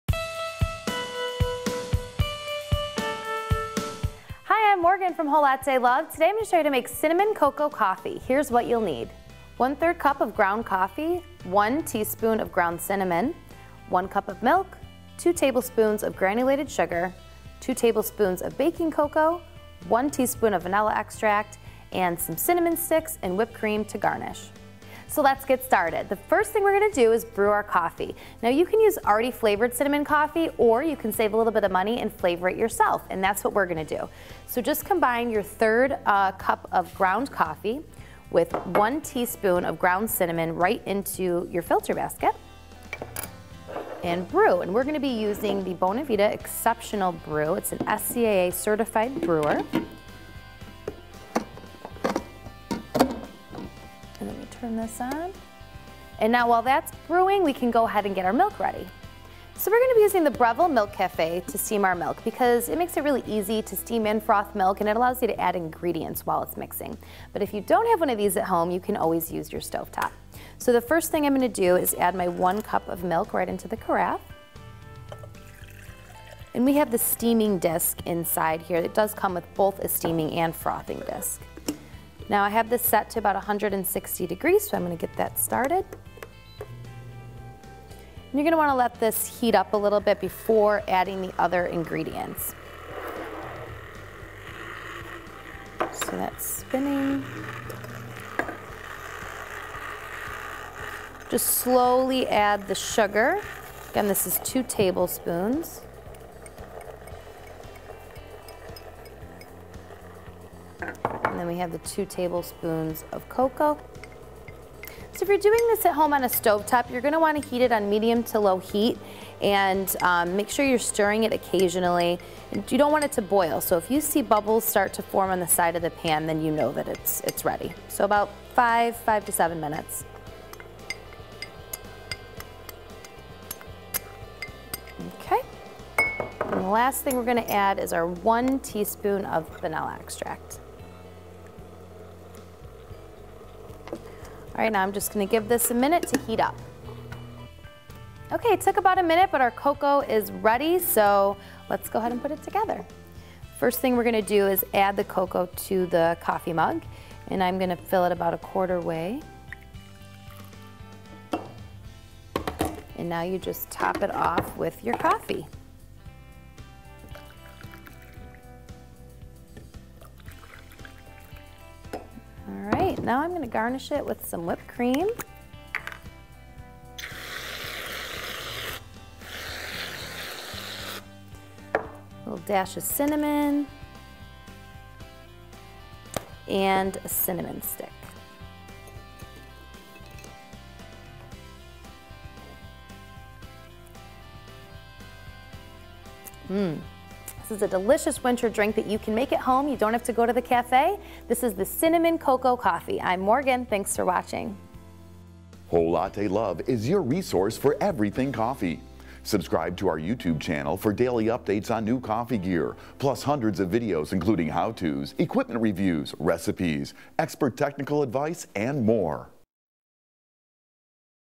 Cinnamon Cocoa Coffee Recipe
Morgan shows you how to make a Cinnamon Cocoa Coffee at home.